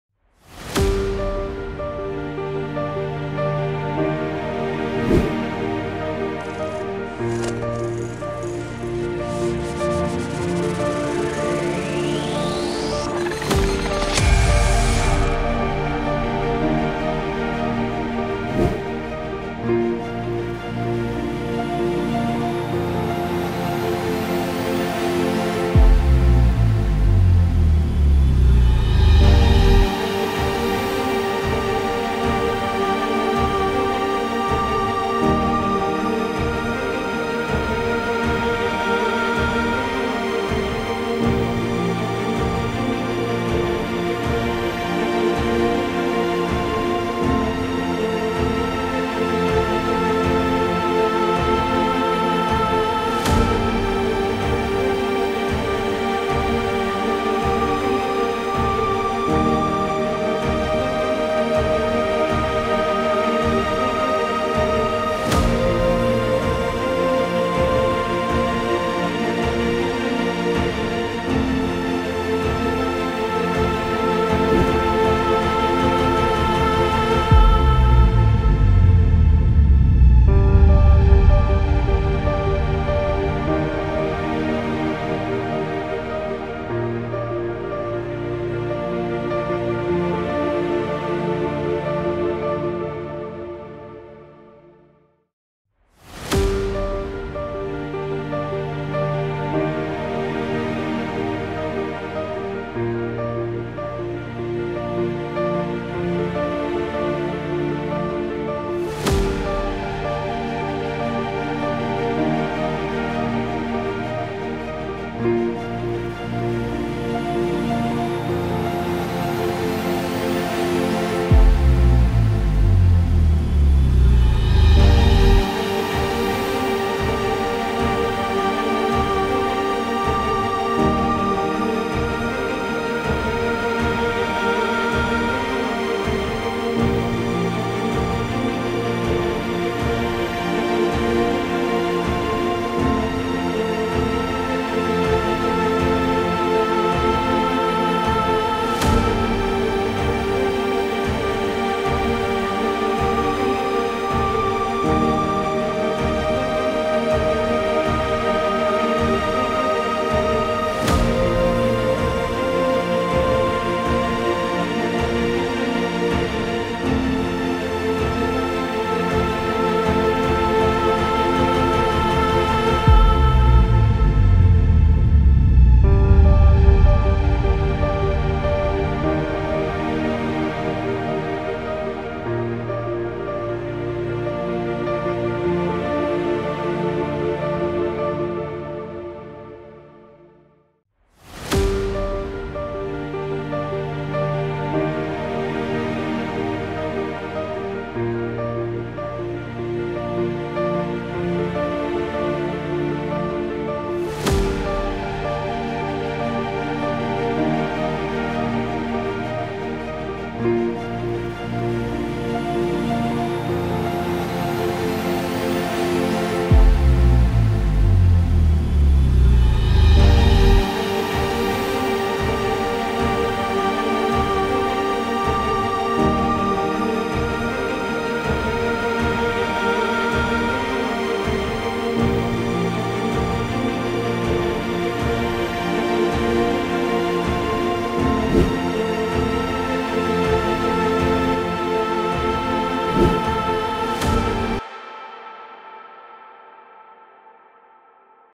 Desert camel speed art |Photoshop manupulation | Beeafter photos
This is a video about photoshop manupulation done by Beeafter photos